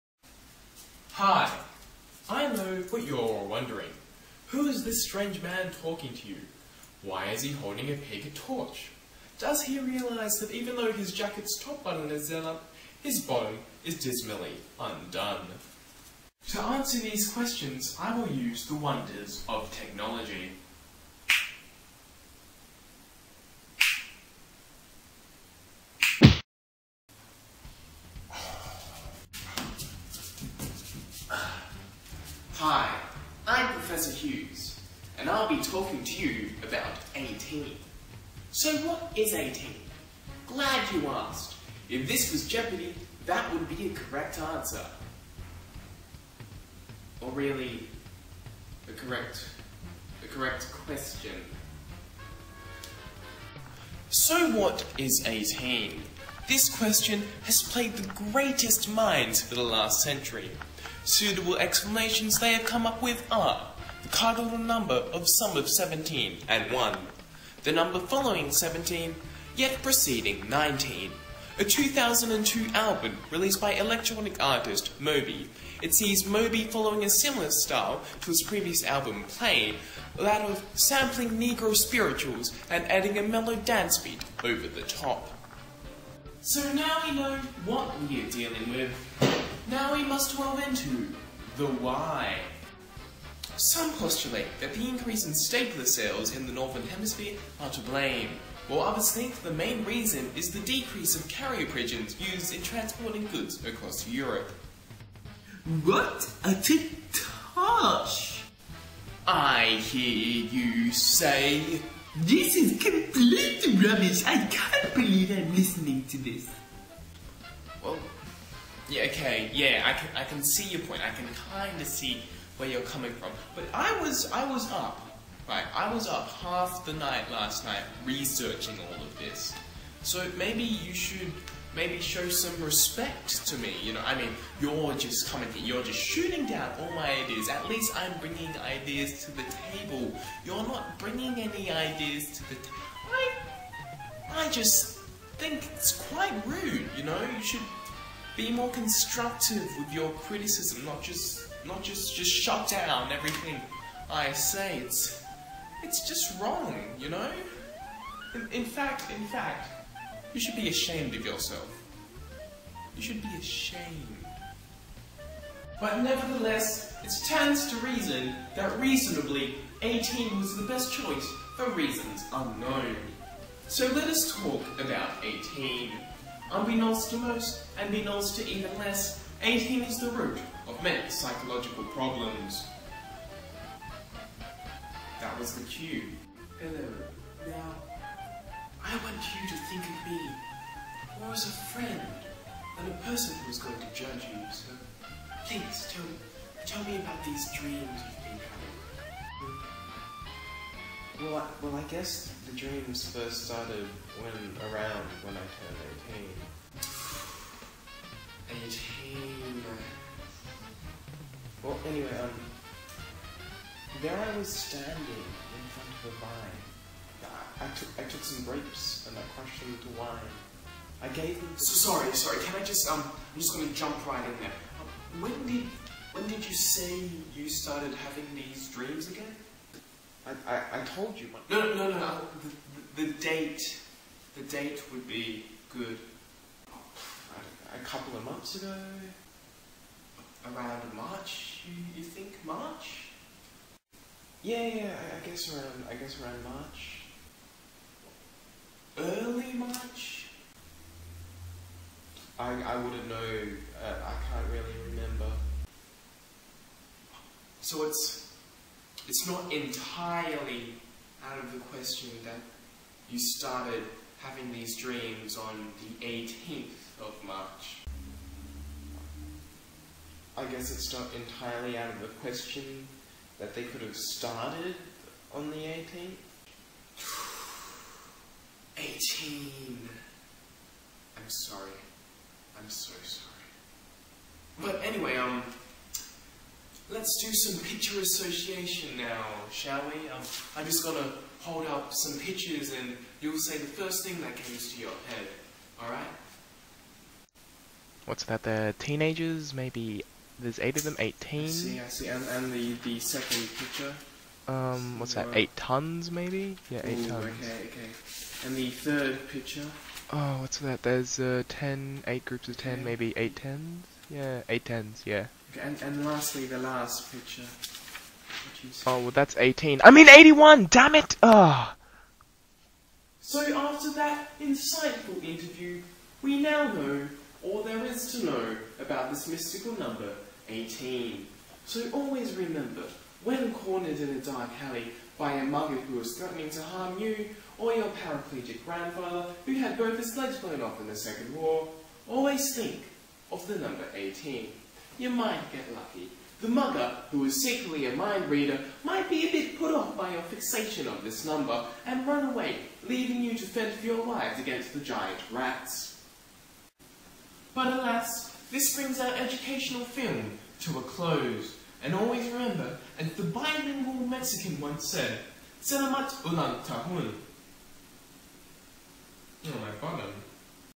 Eighteen
A short movie I made for my friend's birthday card.
It's EDUTAINMENT!
They say that plagerism is the sincerest form of flattery, if so Shaun Micallef should be very flattered right about now. (I swear it is just coincidence that at the time of filming I was watching the Micallef Pogram and that half the jokes are in this video. COINCIDENCE!)